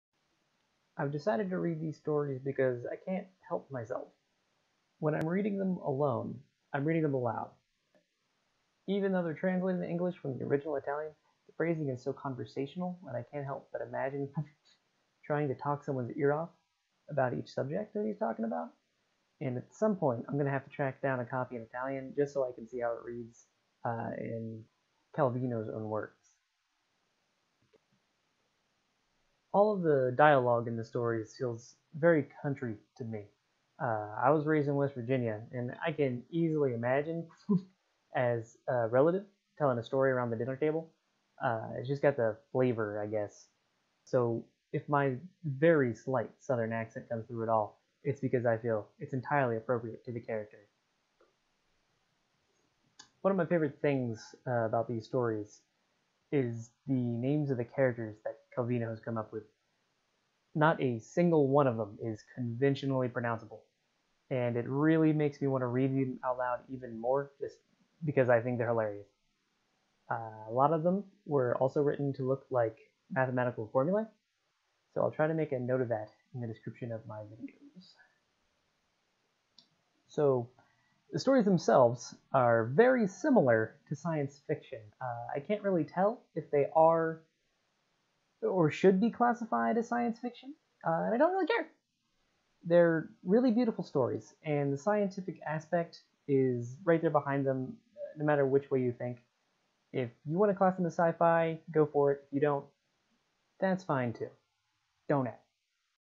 Introduction to The Complete Cosmicomics
I've decided to read Tutte Cosmicomiche to you, the Internet.

Dramatis personæ:
Me: your trusty reader
Qfwfq: a name you shouldn't be able to pronounce